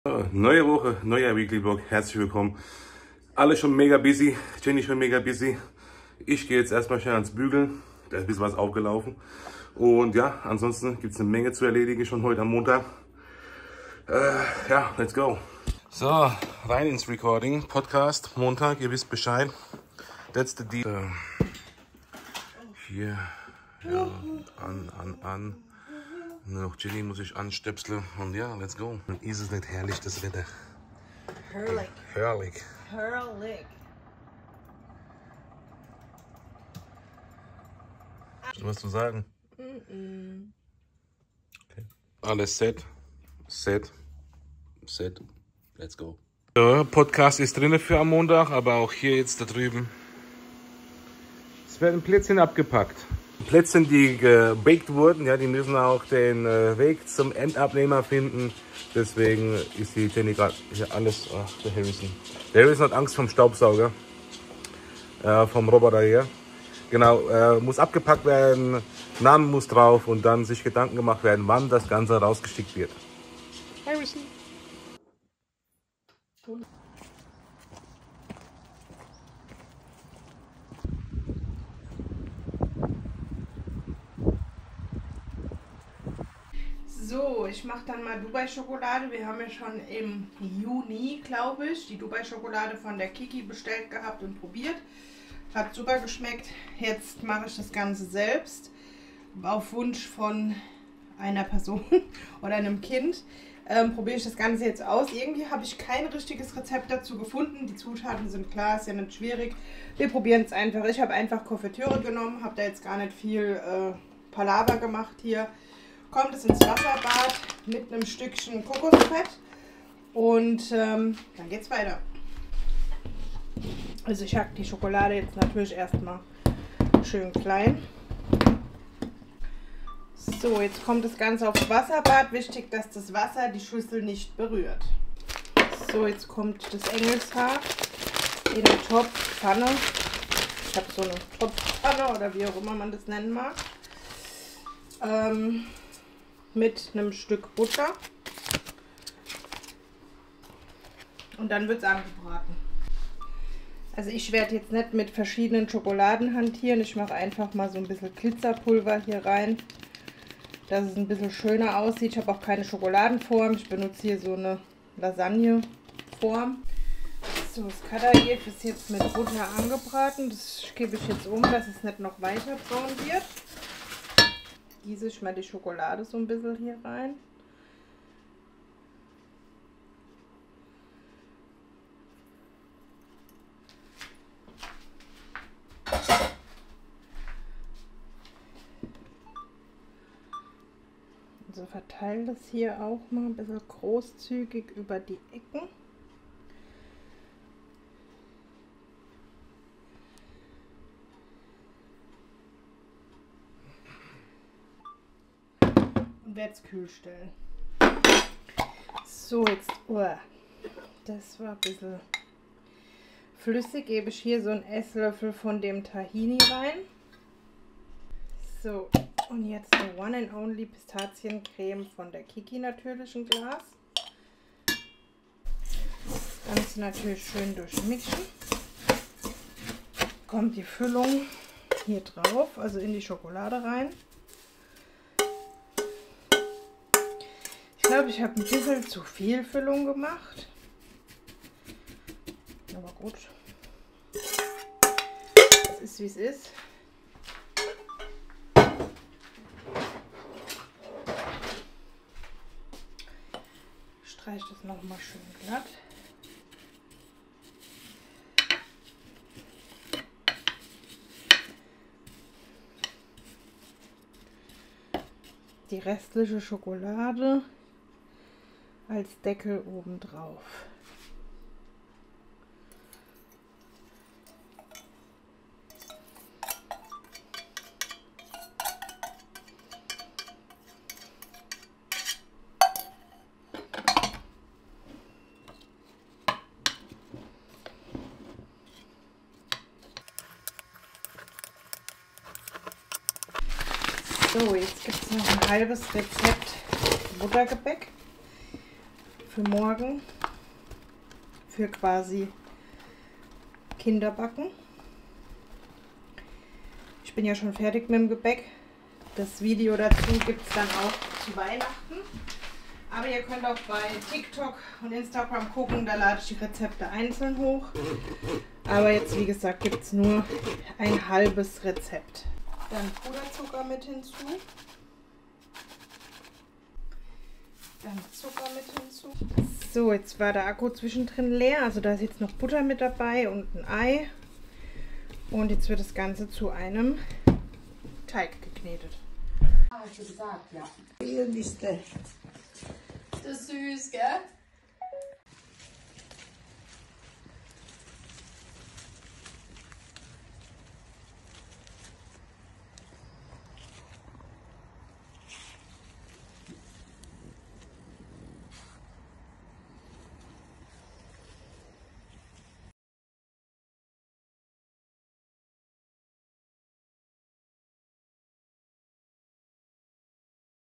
DIY Dubai Schokolade - Don't believe the hype? | Weekly Vlog #37
Taste Test zur Dubai Schoko muss nachgeliefert werden oder nie 😅 im Juni hatten wir bereits die Schoko von der lieben Kiki testen können war ne 10 von 10. Diesmal sind wir aber nicht großartig zum taste testen gekommen. Doma war diese Woche, bzw. ist nach wie vor ziemlich angeschlagen. Die Grippe hatte ihn erwischt, aber vom aller Feinsten. Dennoch haben wir diese Woche ein paar Schnipsel noch zusammentragen können 😍 enjoy und bleibts uns gesund und stabil 😇